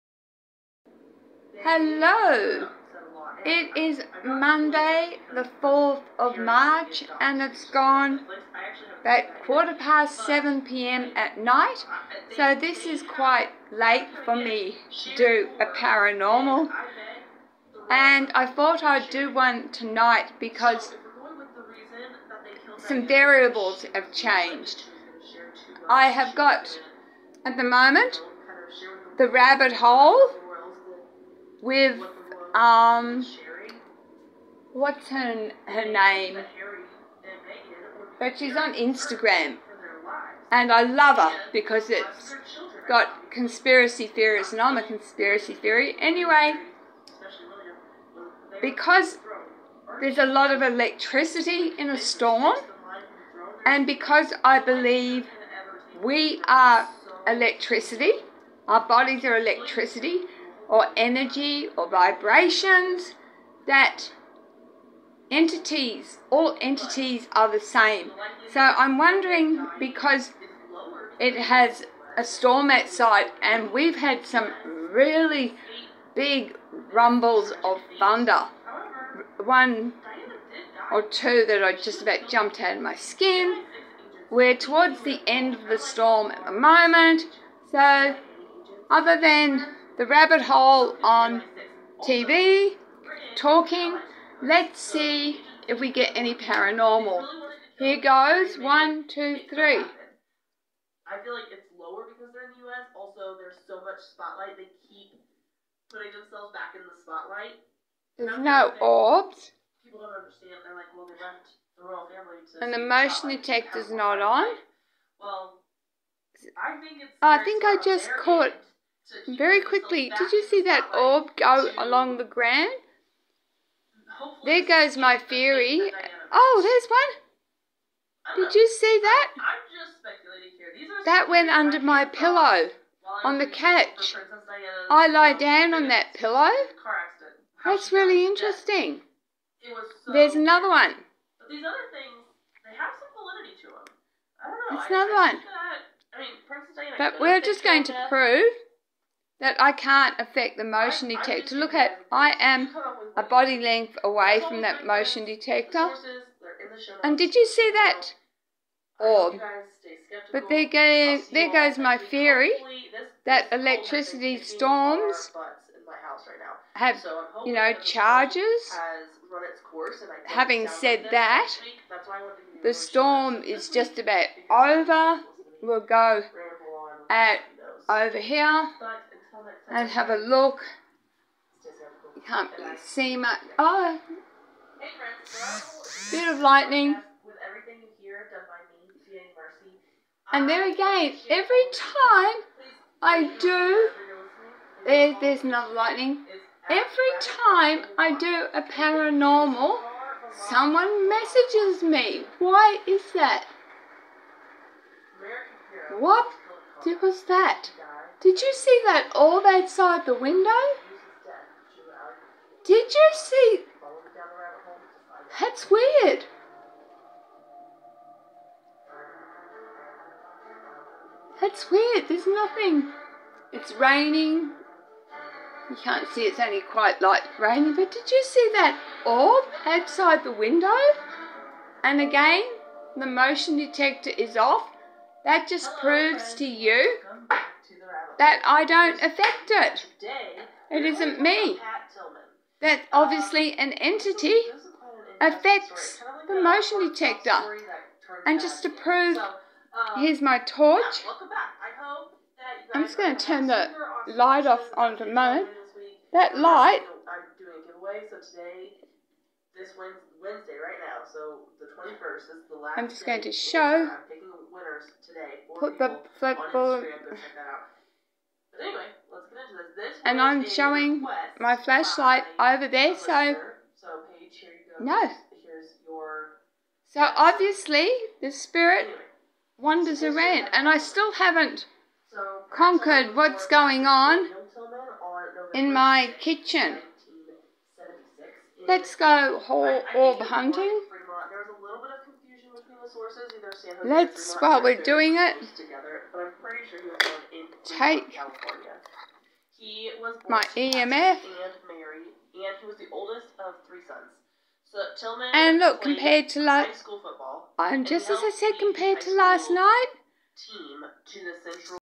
Part 1 while filming something turned off the camera.
Storm tonight. While I was filming at the end the motion detector triggered and something turned my camera off so I continue in part 2.